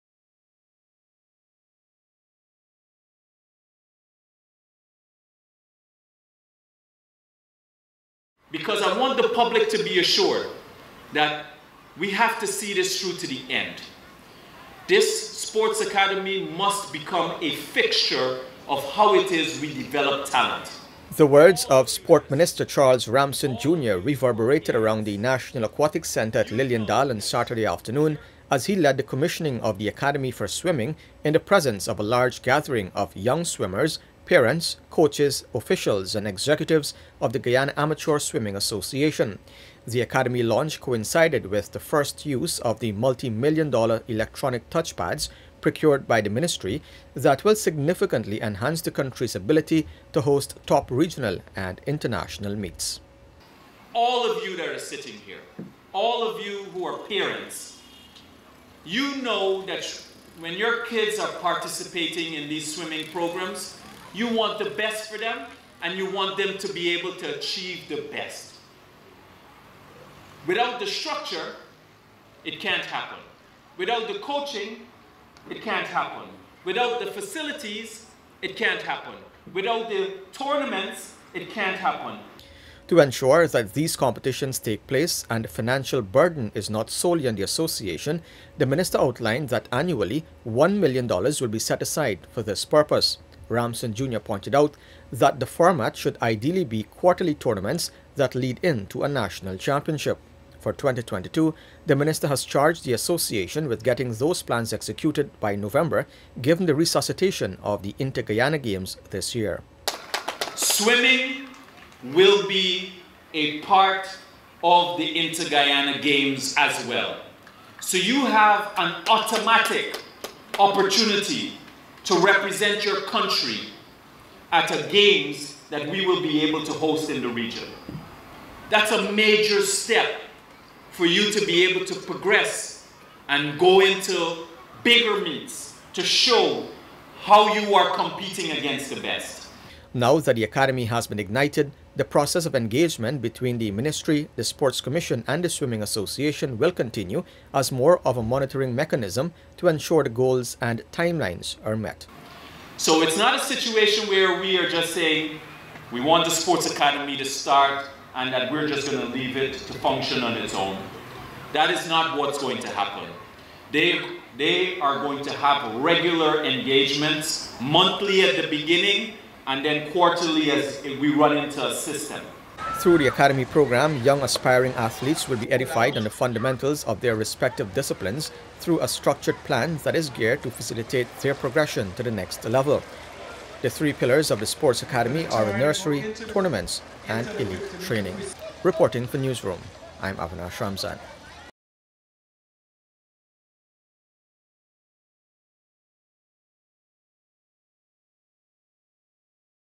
‘BUILDING THE STRUCTURES IS A LONG TERM INVESTMENT’ SPORT MINISTER
The importance of building structures and making long-term investments was underscored at the weekend when the Sport Ministry and the National Sports Commission, in collaboration with the Guyana Amateur Swimming Association, launched the historic Academy swim programme.

The initiative is geared to fast-track the development of the sport from the nursery right up to the elite level.